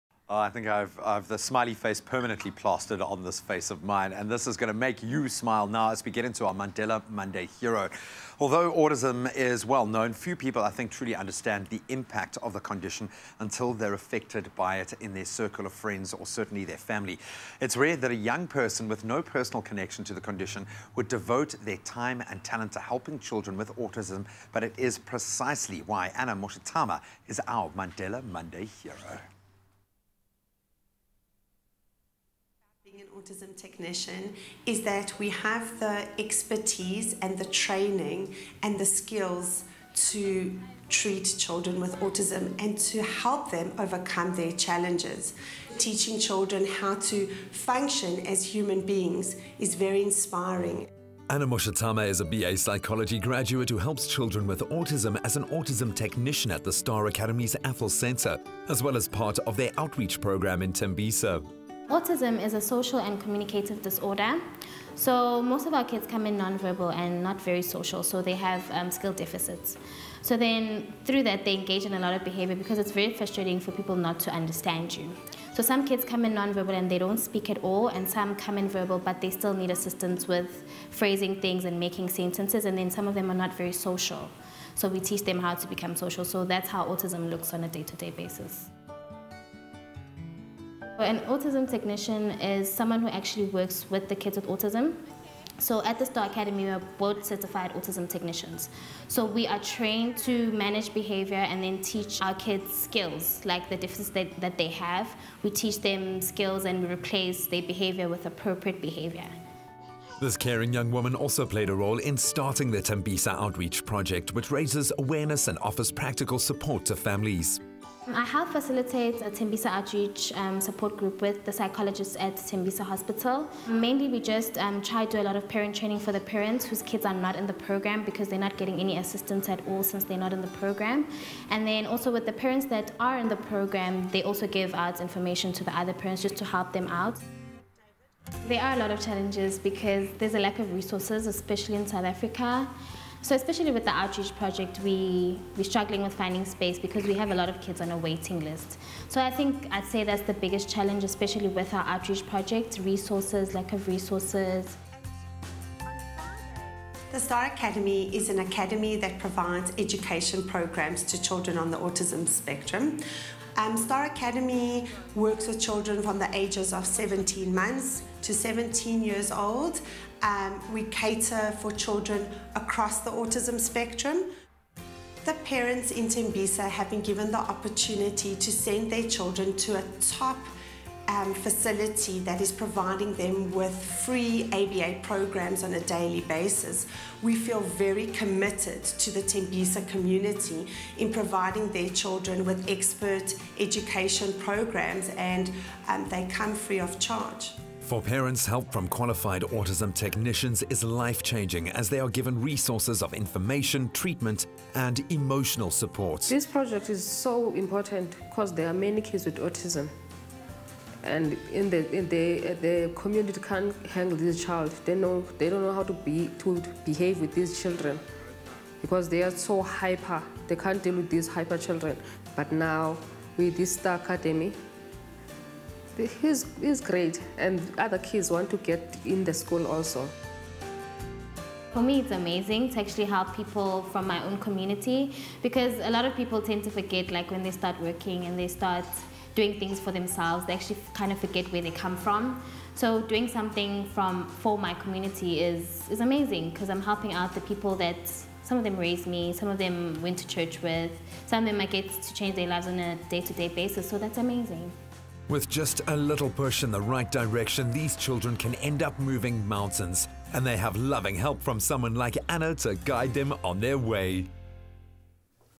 Mandela Monday: Annah Moshatama - Autism Technician | 18 July 2017
Our Mandela Monday Hero is a young autism technician helping children in need.